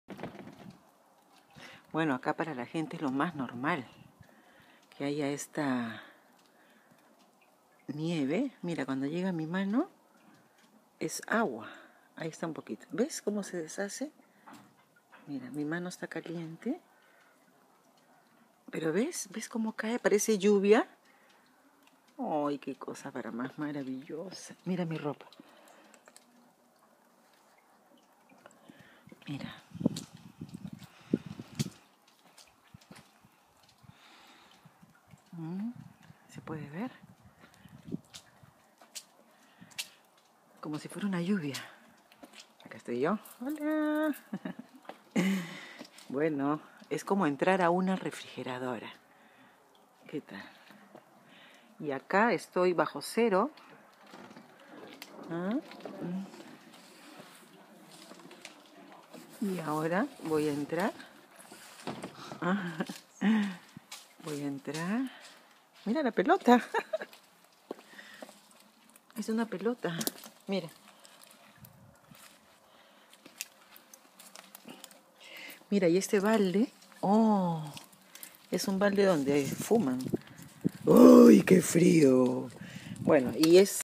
Nieve en Alemania empezó 23/11/15
Mira el proceso, se llena todo de nieve, mira una pelota y un balde con nieve todo se congela brrrrr